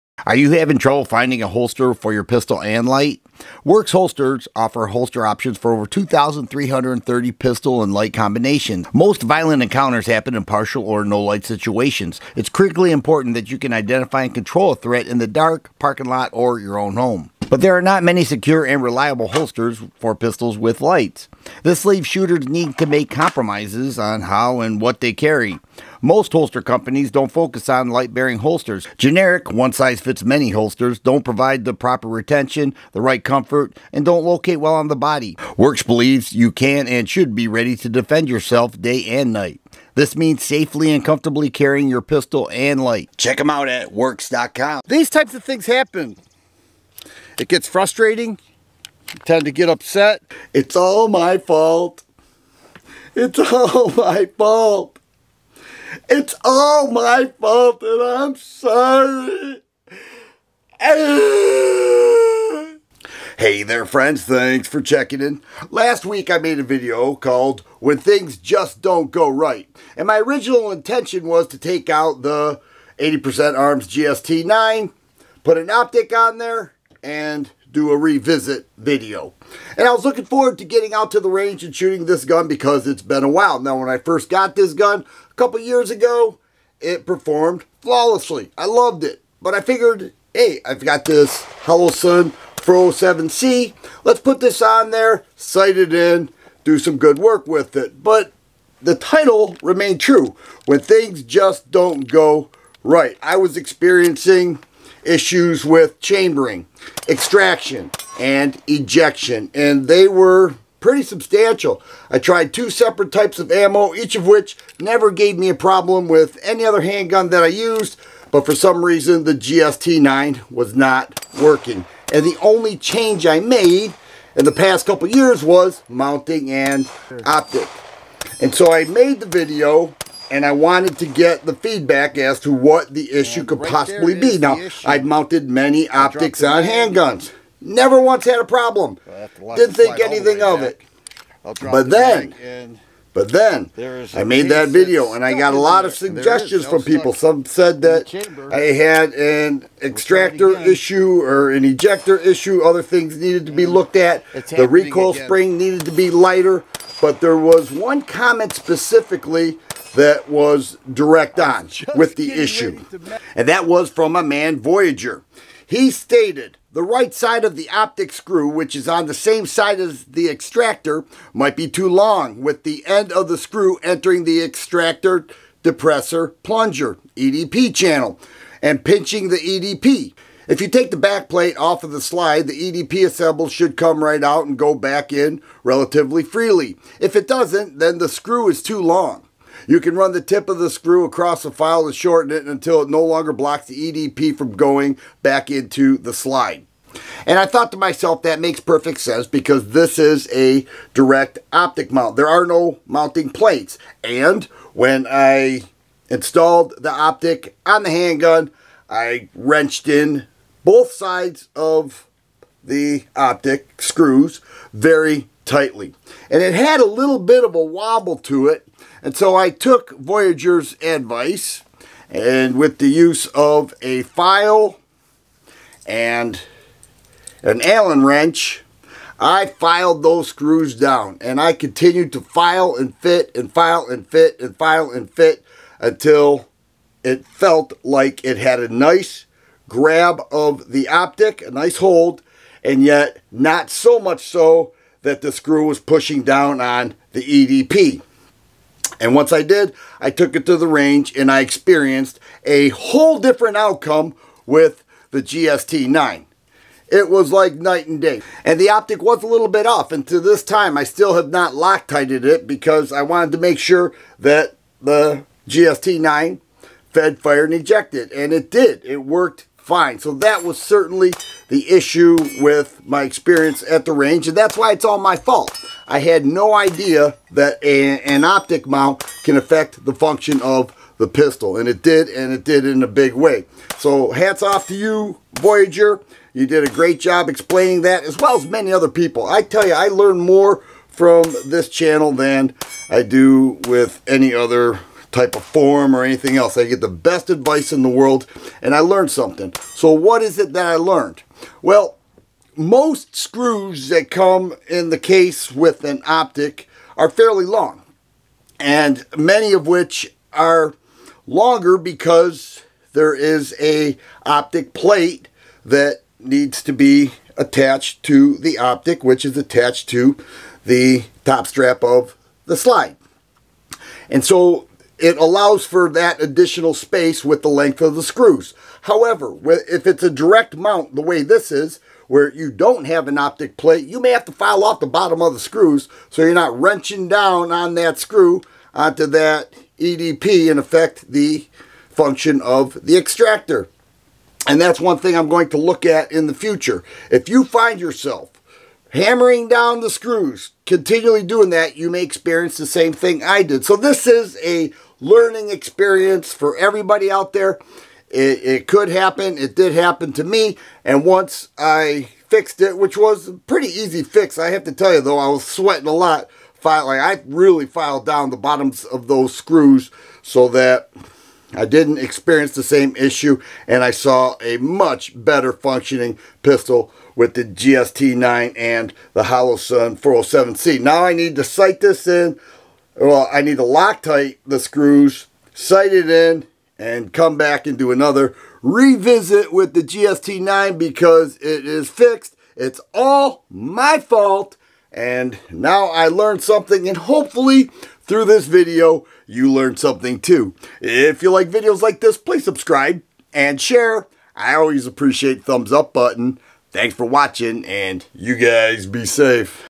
IT"S ALL MY FAULT!! - TheFirearmGuy
It's All My Fault!! In this video, I explain what the problem was with the GST9. I consider this a learning experience for me that may help someone in the future.